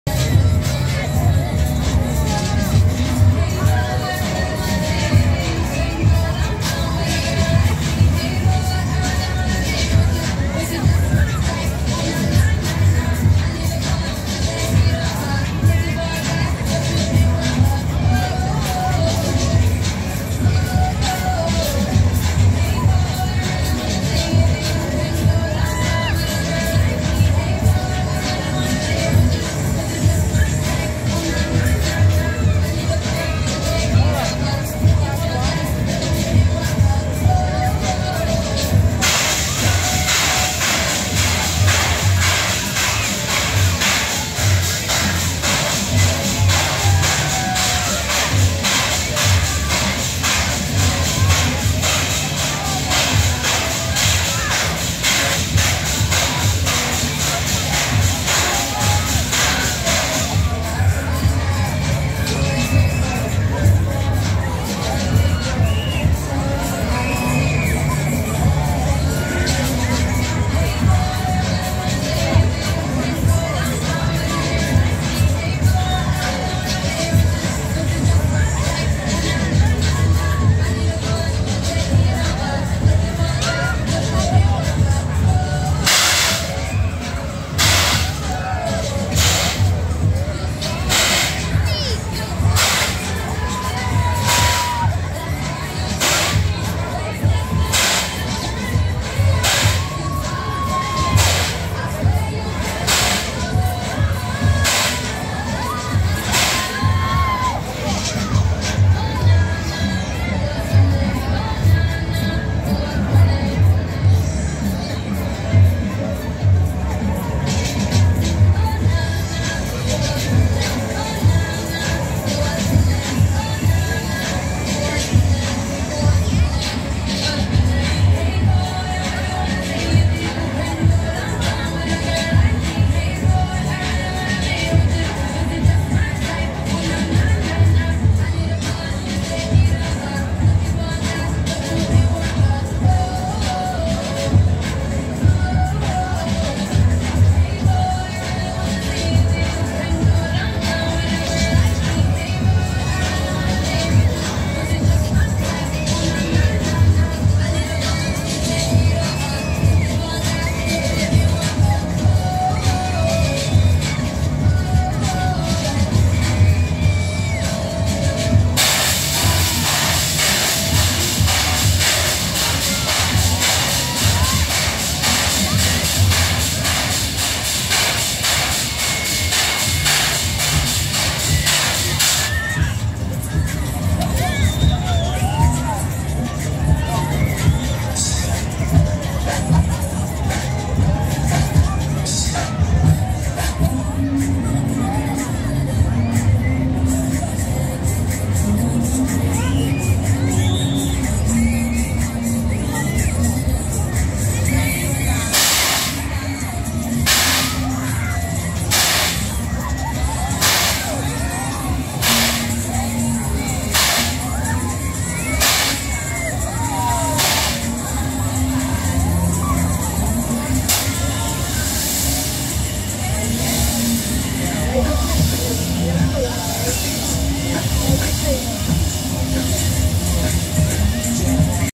BOUNCE At Blackheath Common Fun Fair/Off-Ride
Owner:
Manufacturer: Safeco
Off ride footage of the Jump & Smile at Blackheath Fun Fair.